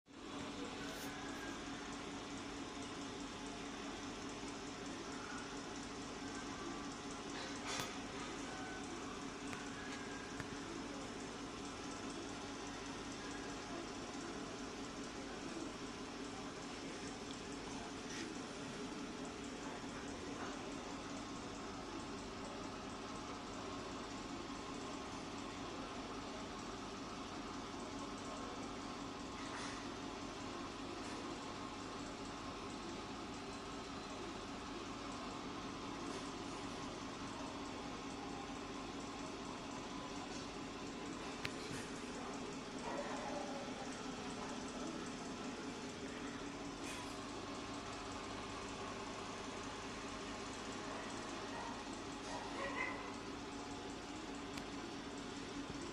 Tank in summer 2015
Somehow fans kept it under 30 degrees but lucky enough to have kept hardy plantsstem